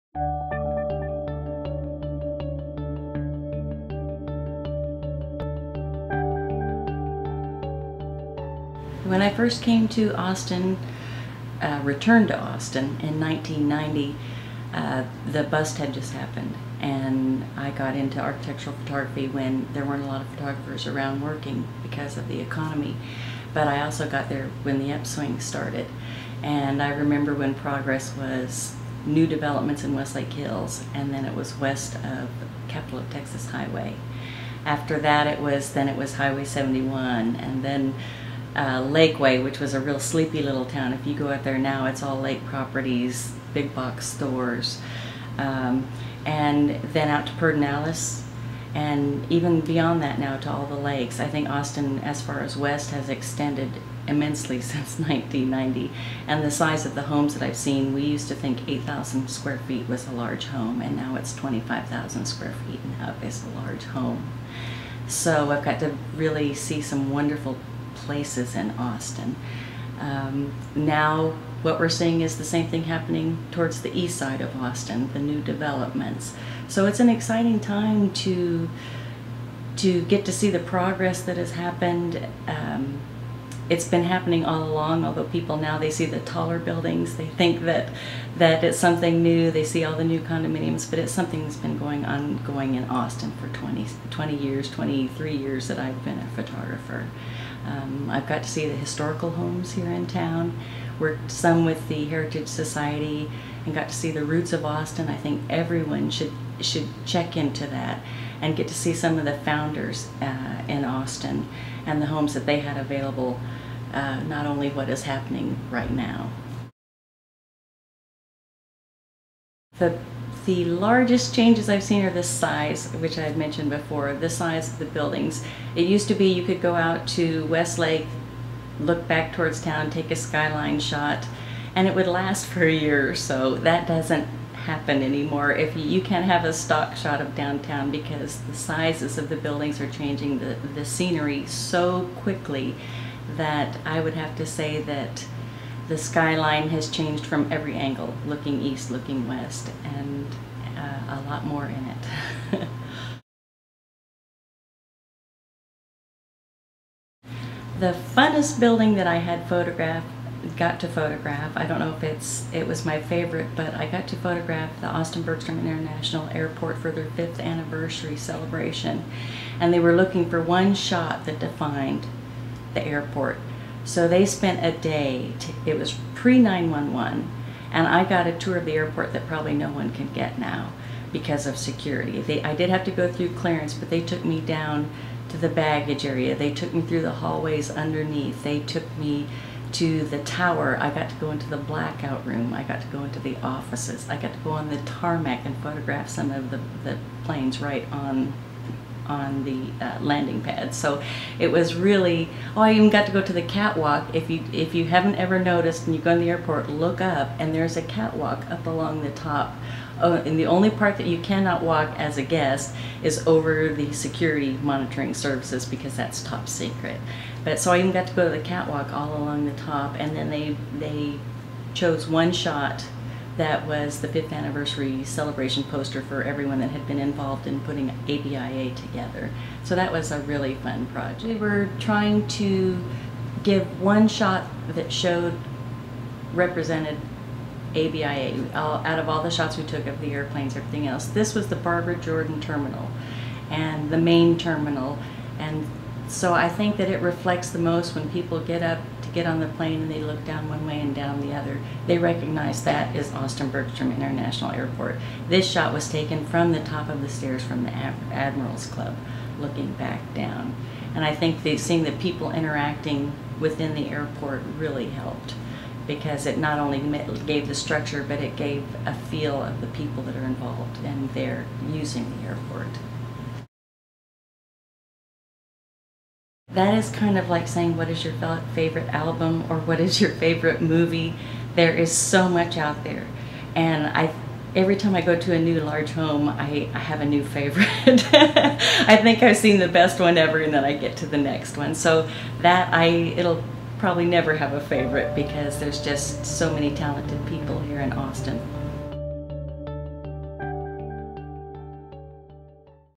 Architecture from a photographer's point of view-Interview with Tina Weitz
Tina Weitz has been photographing residential and commercial projects in Austin since the 1990s, and in this interview, she tells of the changes shes seen in Austin and gives a peek at some of the photos shes taken over the years.